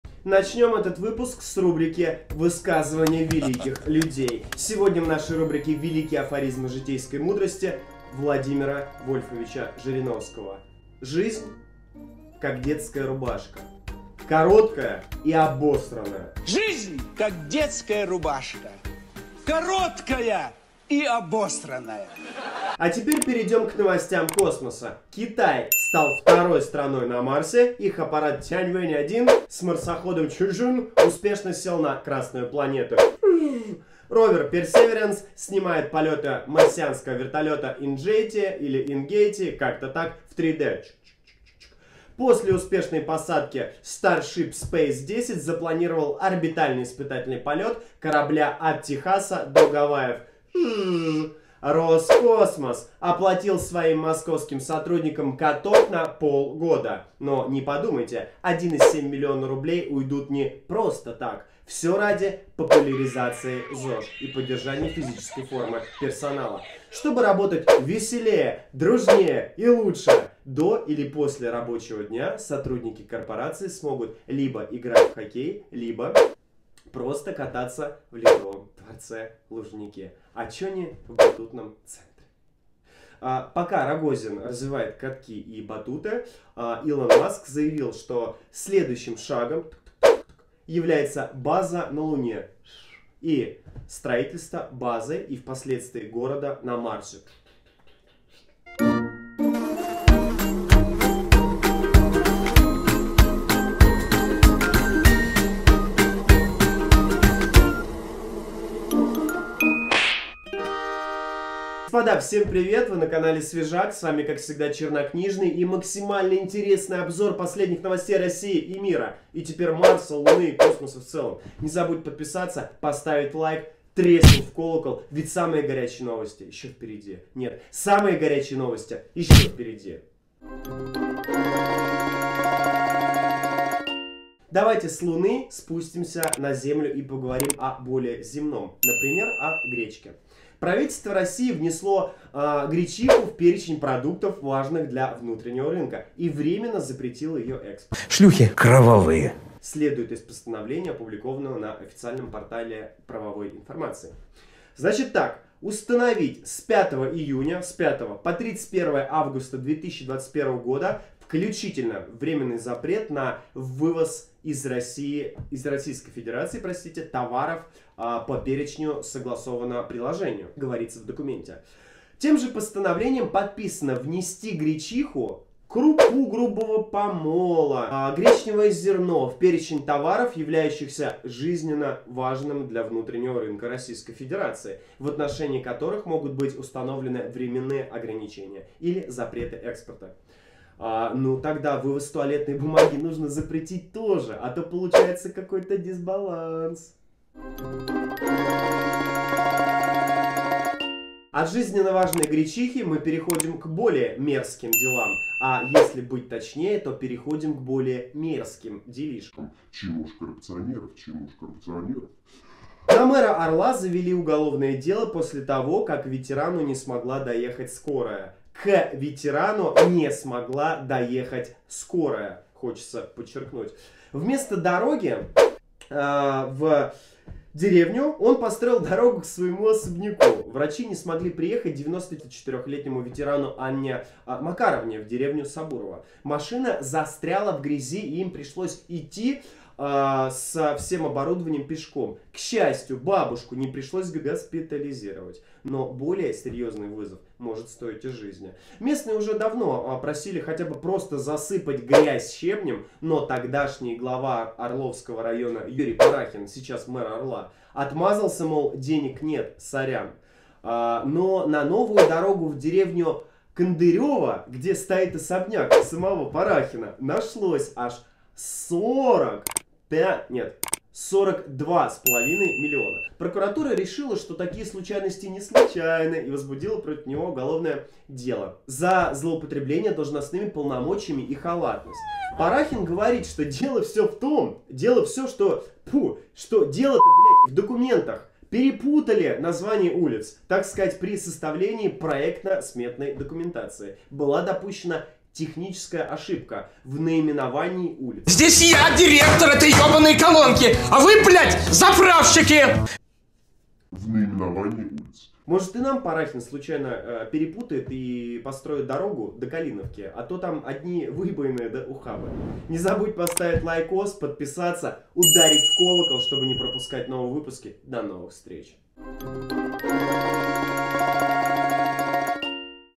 Китай высадился на Марсе \ Запрет на вывоз гречки из России / Скорая так и не приехала
Китай высадился на  Марсе / Запрет на вывоз грече из России / Скорая так и не приехала / Чернокнижный обзор интересных событий России и Мира!

Тайм-коды:

00:00 - Подписывайся на Свежак

00:16 - Жириновский о жизни
00:53 - Каток для сотрудников Роскосмоса
01:27 - Илон Маск о строительстве города на Марсе
02:14 - Правительство запретило экспорт гречки
03:21 - Скорая из-за отсутствия дороги не смогла приехать к ветерану ВОВ
05:15 - Подписывайся на СВЕЖАК